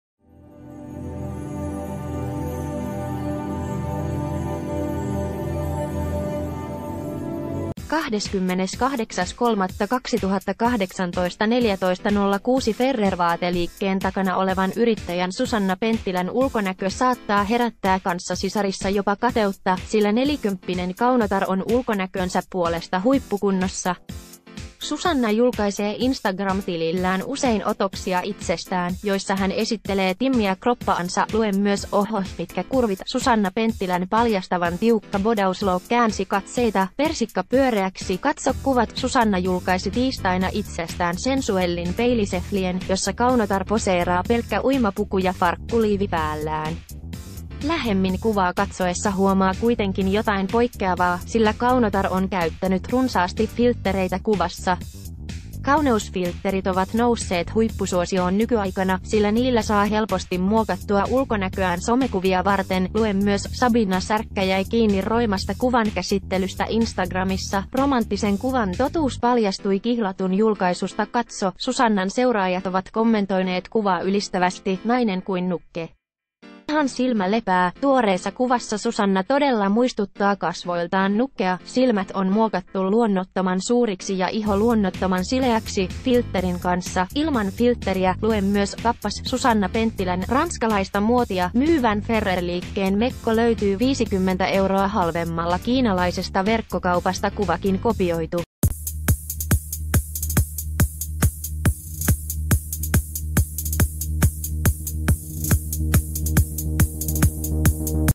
Viimeisimmät uutiset | Nyt meni filtterit överiksi! Susanna Penttilä muokkasi selfiensä aivan luo...
Viimeisimmät uutiset | Nyt meni filtterit överiksi! Susanna Penttilä muokkasi selfiensä aivan luonnottomaksi - katso kuvat!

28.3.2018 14:06 Ferrer-vaateliikkeen takana olevan yrittäjän Susanna Penttilän ulkonäkö saattaa herättää kanssasisarissa jopa kateutta, sillä nelikymppinen kaunotar on ulkonäkönsä puolesta huippukunnossa. Susanna julkaisee Instagram-tilillään usein otoksia itsestään, joissa hän esittelee timmiä kroppaansa.  Lue myös:  OHHOH, MITKÄ KURVIT! SUSANNA PENTTILÄN PALJASTAVAN TIUKKA BODAUSLOOK KÄÄNSI KATSEITA: "PERSIKKA PYÖREÄKSI" - KATSO KUVAT! Susanna julkaisi tiistaina itsestään sensuellin peilisefli...